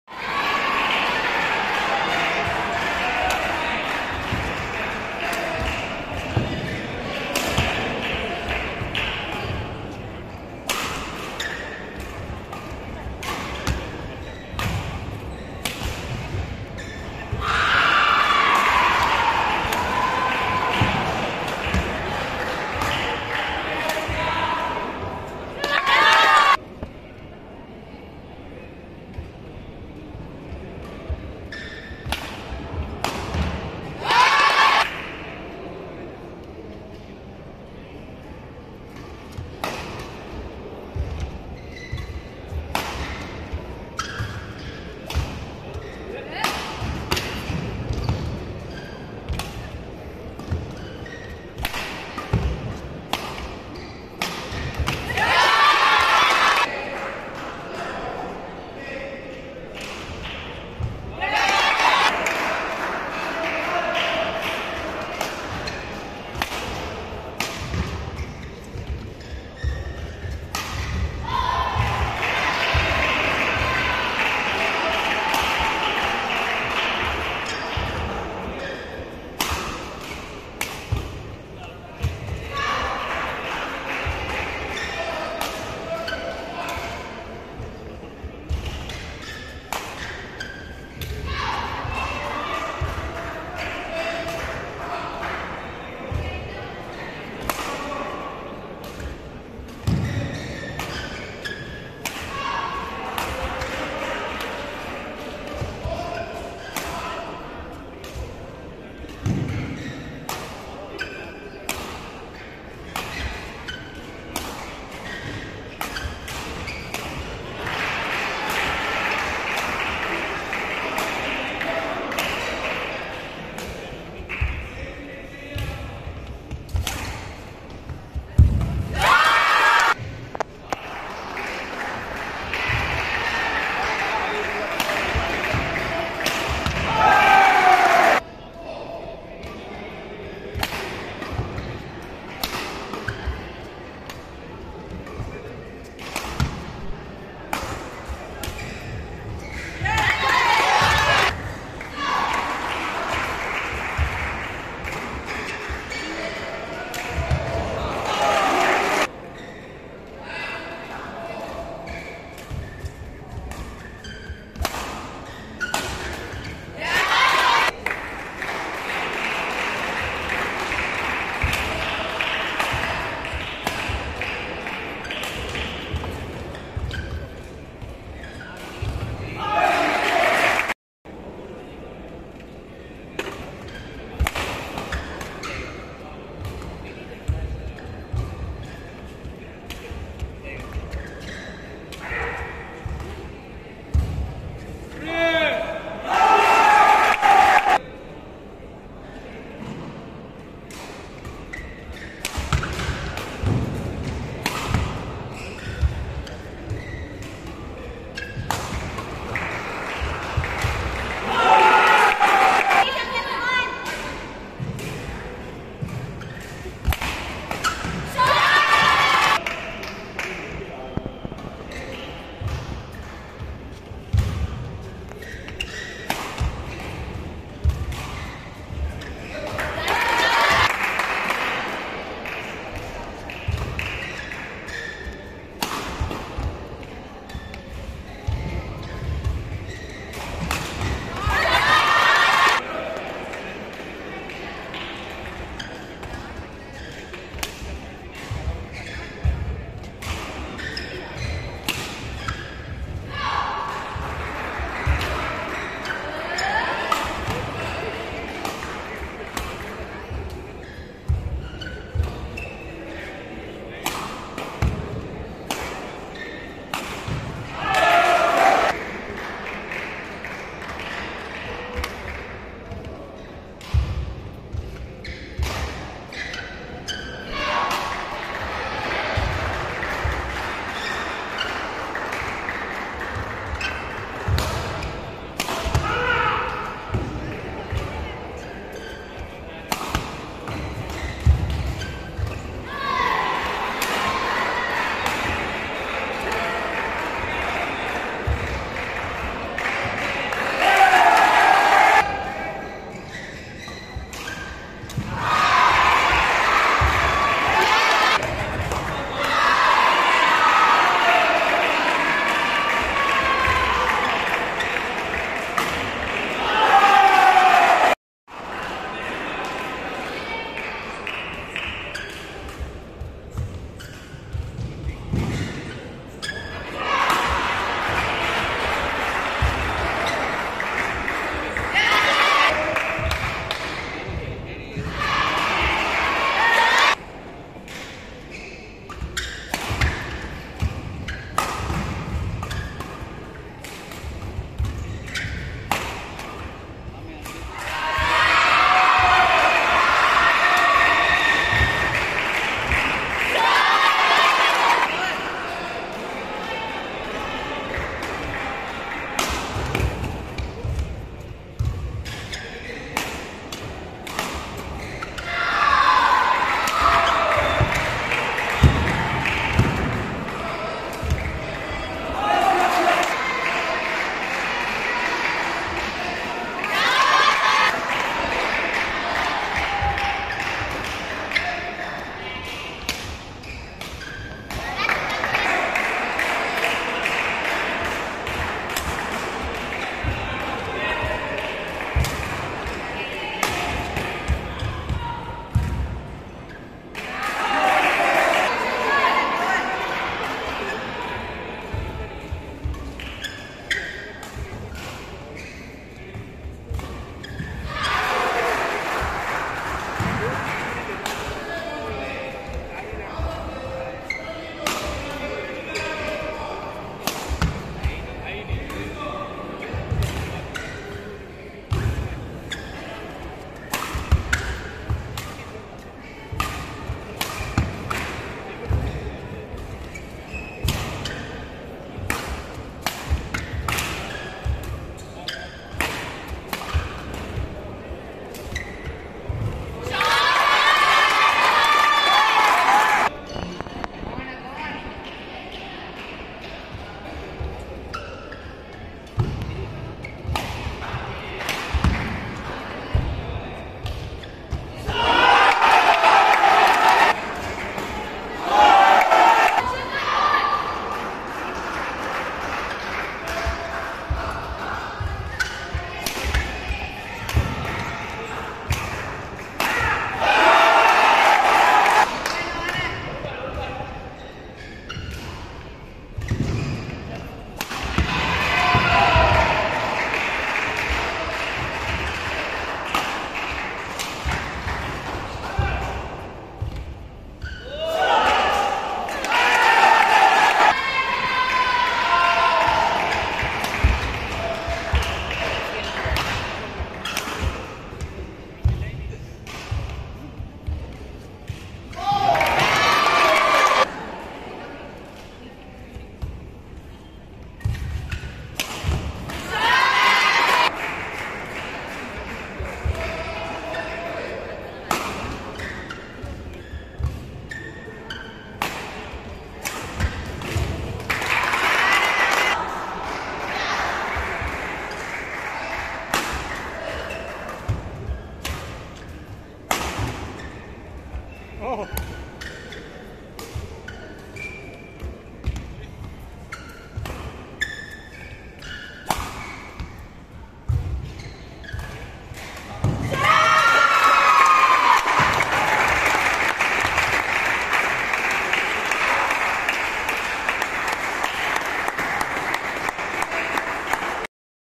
SHAKAR MUTHUSAMY Vs BHARATH LATHEESH : UNDER 19 SOUTH ZONE INTER STATE BADMINTON CHAMPIONSHIP 2022
76th YONEX SUNRISE SOUTH ZONE INTER STATE BADMINTON CHAMPIONSHIP 2022

This video shows the UNDER 19 Singles match between SANKAR MUTHUSAMY Vs BHARATH LATHEESH.